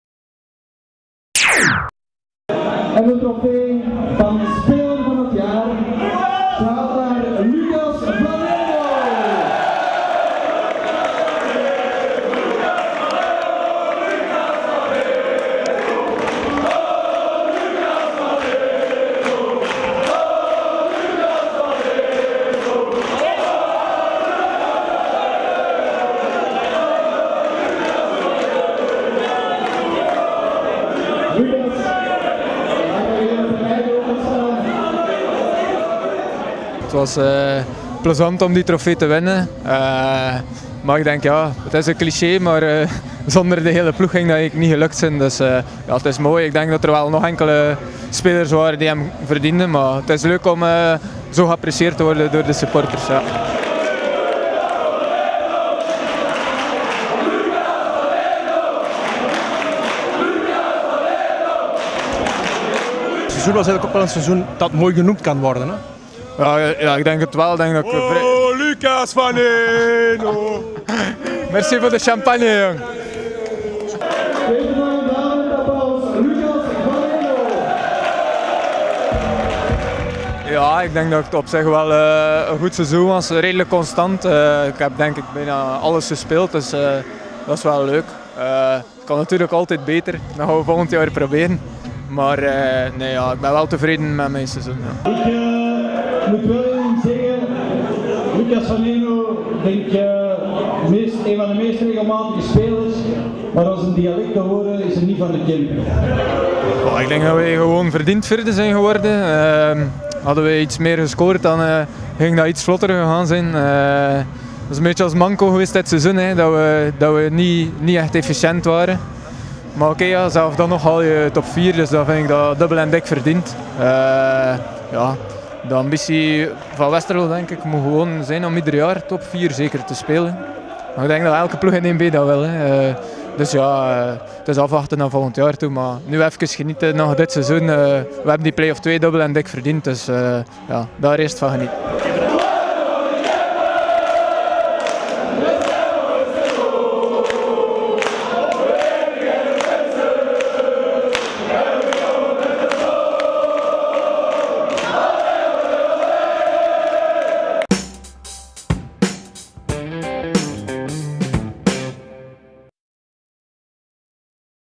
Kemphaan van het seizoen: Lukas Van Eenoo (kvcTV - 27/04/19)
Tijdens het jaarlijkse supportersbal werd de 'Kemphaan van het seizoen' verkozen.  Lukas Van Eenoo ging met de trofee aan de haal.  De sfeer zat er duidelijk in en de champagne vloog rijkelijk in het rond.